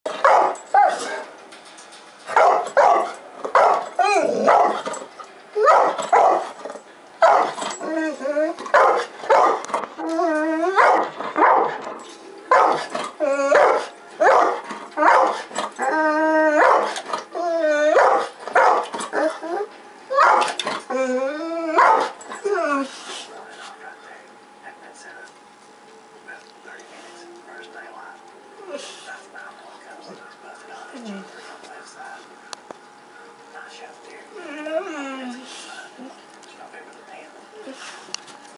bullmastiff watches deer hunting shows with me..
Bullmastiff  Starmie, ,   likes to watch deer hunting shows on TV . I cant watch a deer hunting show without her trying to attack  the tv..lol